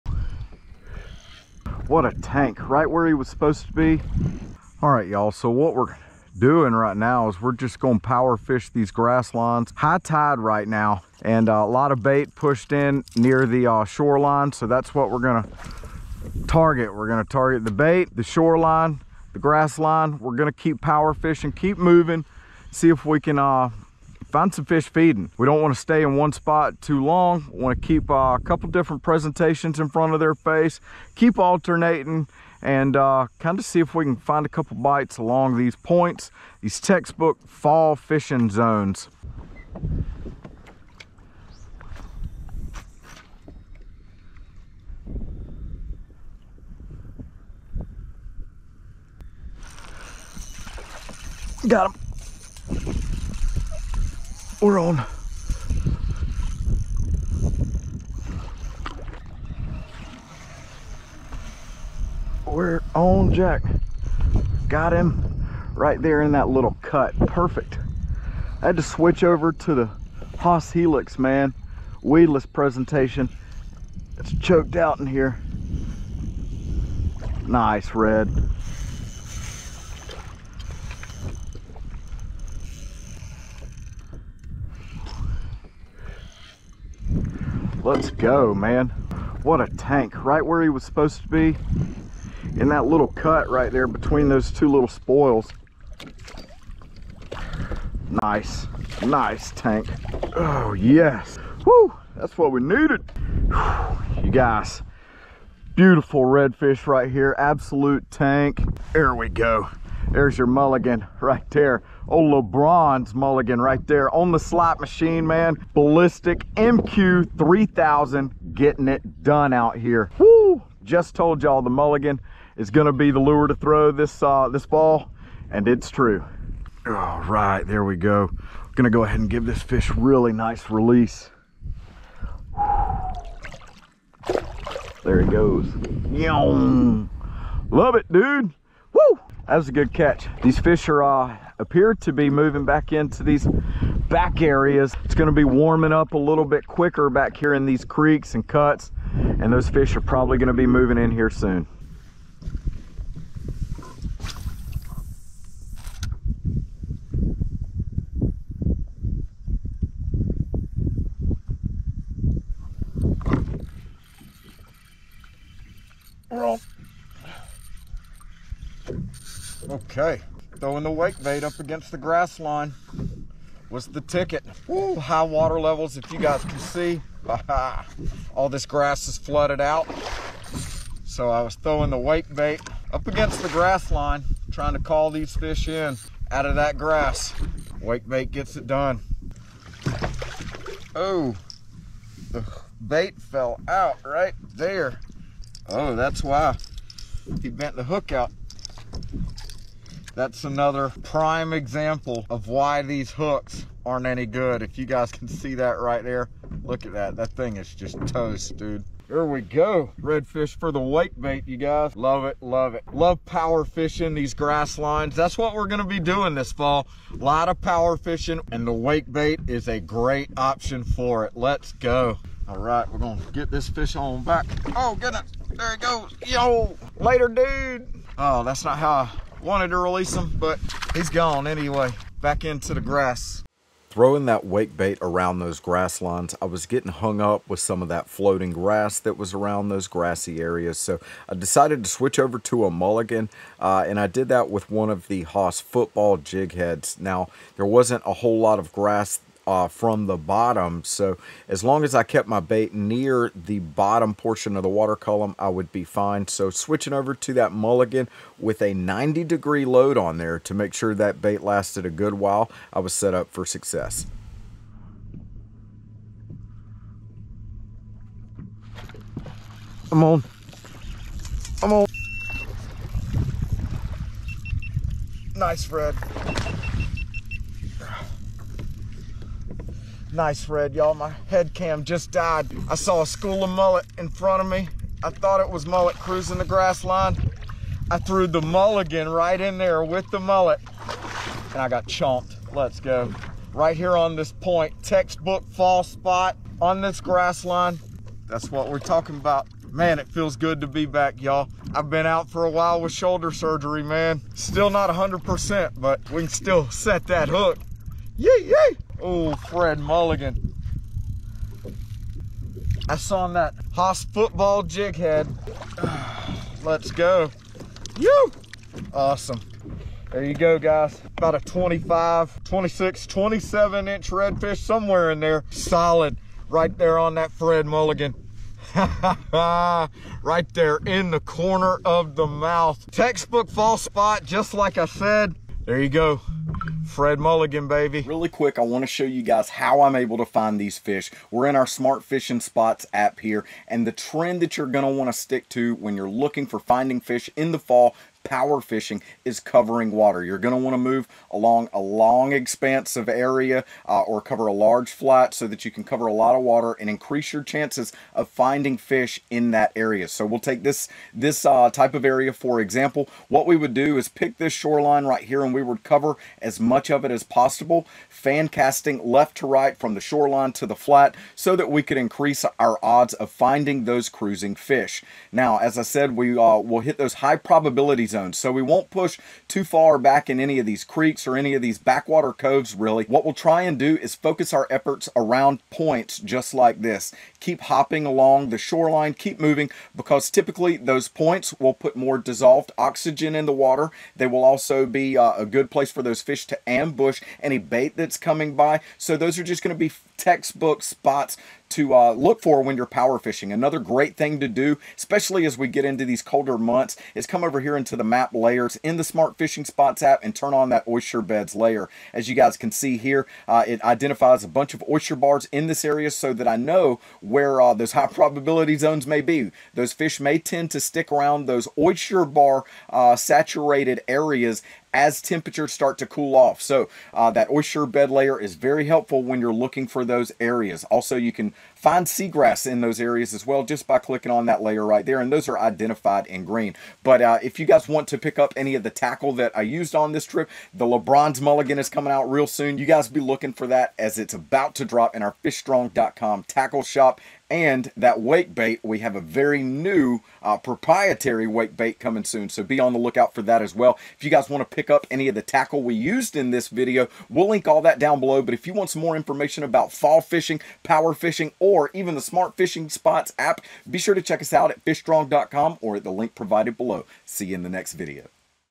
Expert Power Fishing Tactics To Catch Inshore Slams In Late Fall
The key to finding the bite in late fall: POWER FISHING.

Don’t stay in one spot for too long.

Buzzing baits down shorelines looking for bait is a surefire way to find the bite.

Keep moving and have multiple presentations at the ready.

Change lures and locations to find the bite.

Want to start using our software to catch more fish?